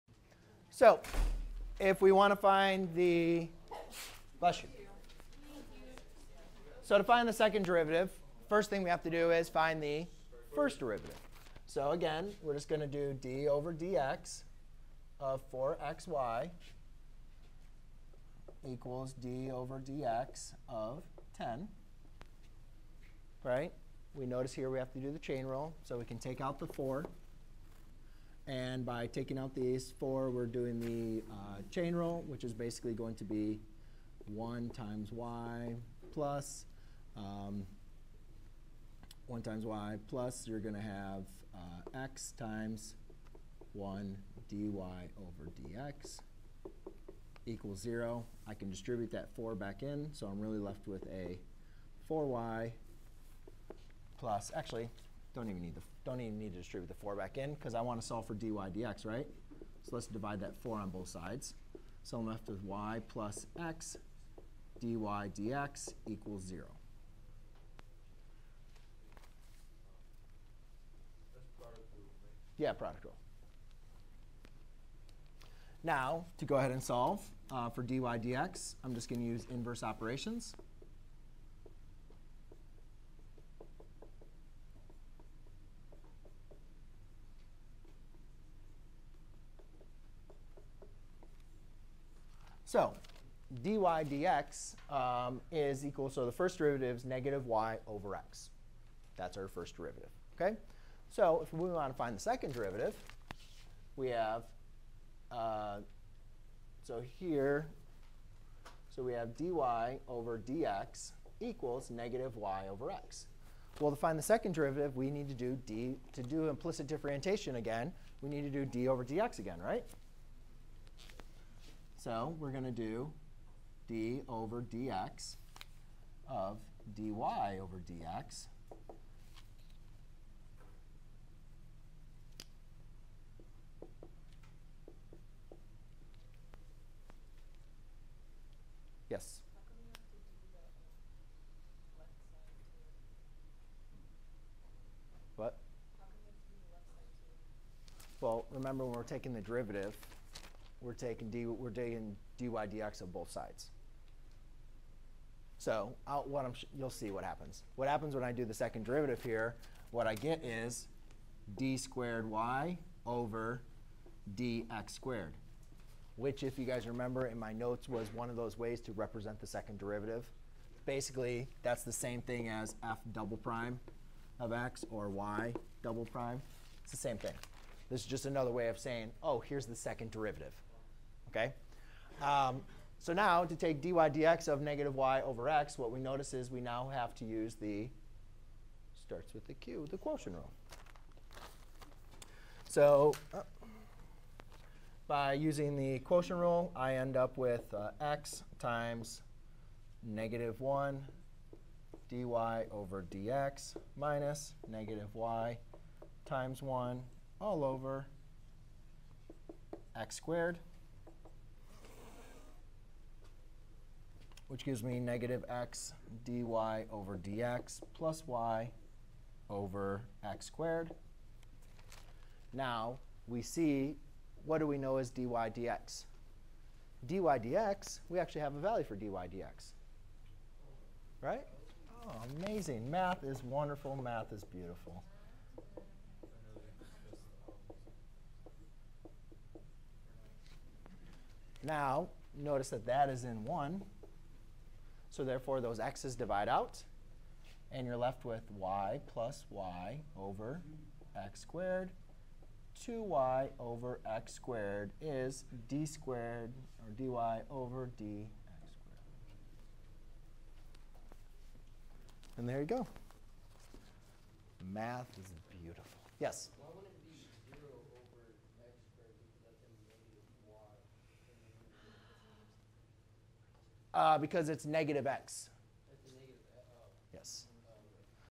How to take the second derivative using implicit differentiation
👉 Learn how to find the derivative of an implicit function. The derivative of a function, y = f(x), is the measure of the rate of change of the function, y, with respect to the variable x. The process of finding the derivative of a function is called differentiation.
A function is said to be implicit if the dependent variable is not explicitly defined in terms of the dependent variable. To differentiate a function in terms of another variable not used in the function, we differentiate both sides of the equality sign and then make use of the sum, product, and quotient rules of differentiation as the case may be to complete the differentiation.